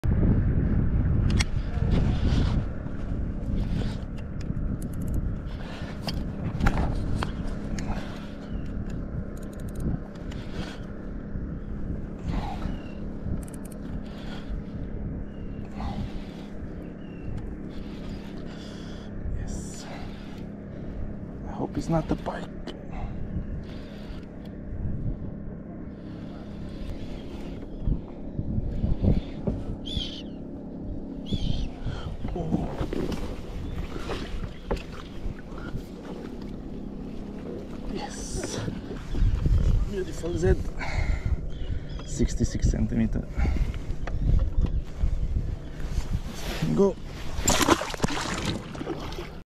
BIG reservoir kayak fishing
Lure fishing for zander